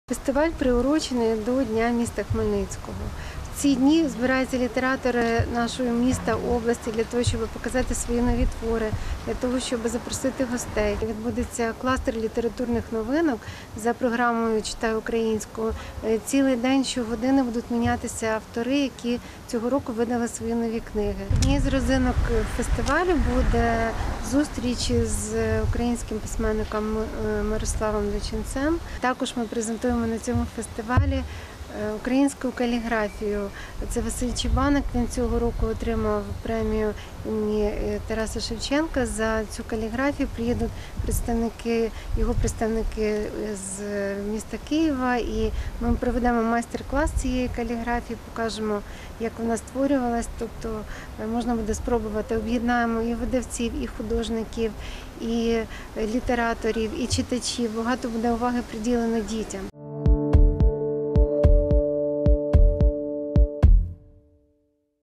Літературний фестиваль «Слово єднає – 2019!» відбудеться в Хмельницькому
Літературний  фестиваль  «Слово єднає – 2019» вчетверте відбудеться в обласному центрі. Розповідає директор Хмельницької міської централізованої бібліотечної системи Тамара Козицька. За її словами, цього року фестиваль буде масштабнішим ніж у попередніх роках, будуть залученні різні локації міста. Триватиме фест з 16-го до 28 вересня.